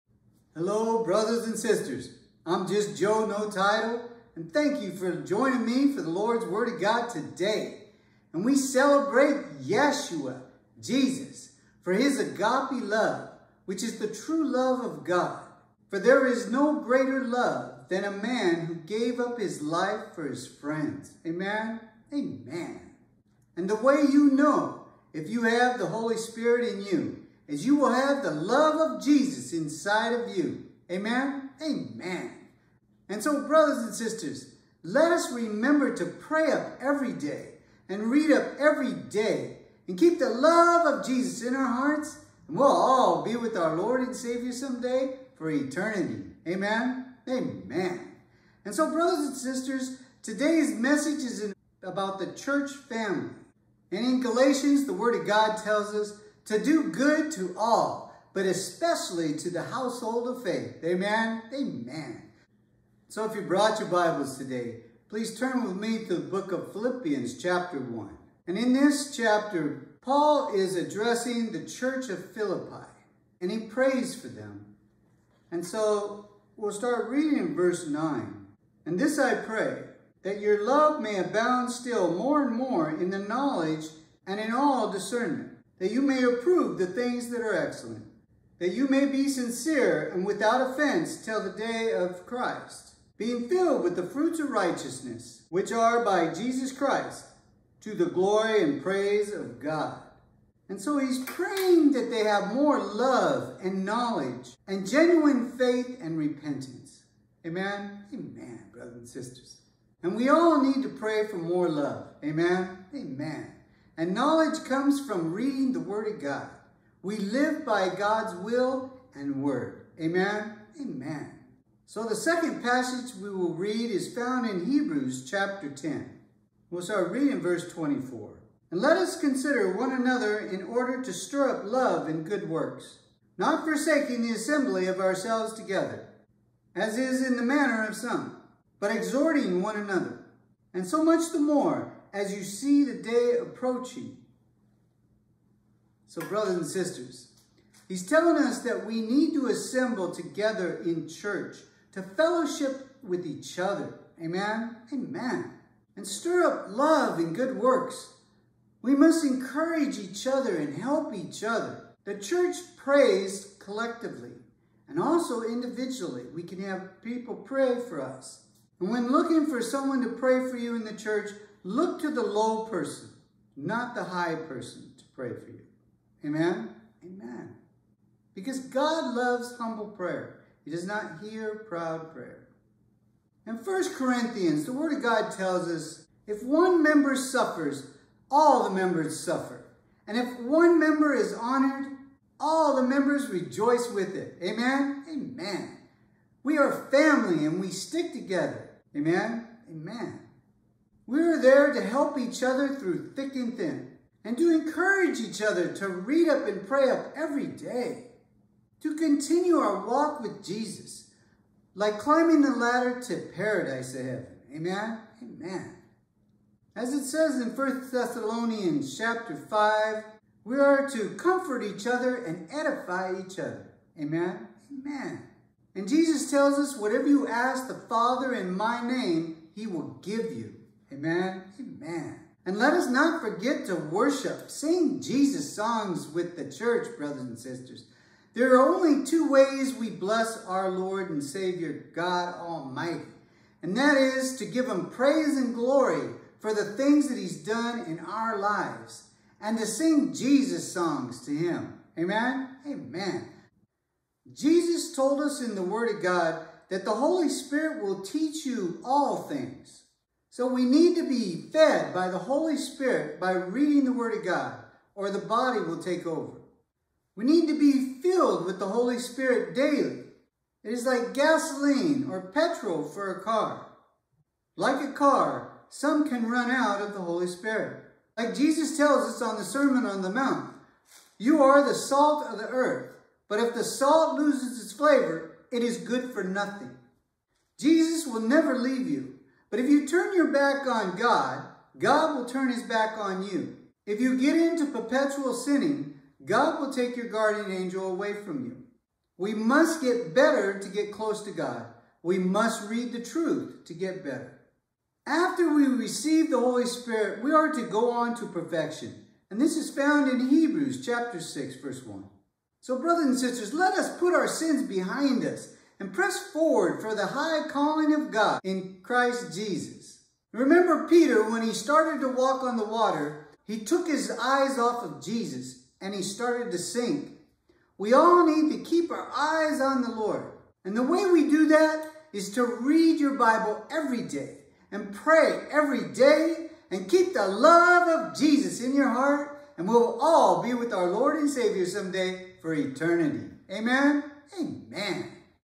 The Church Family@JustJoeNoTitle
I am Just Joe No Title, by the guidance and direction of the Holy Ghost, preaching about the importance of attending church.@JustJoeNoTitle